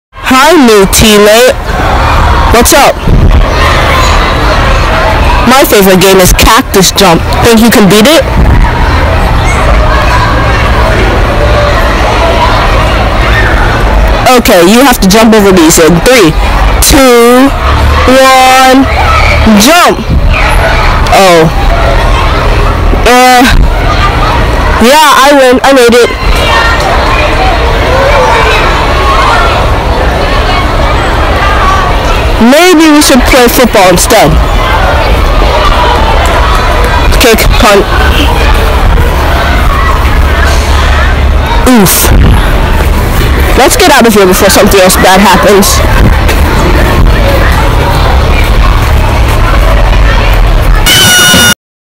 TM2E 1: MiniMonkey’s Bond
MiniMonkey must bond with his teammate,Jayden. However,is it as easy as it sounds?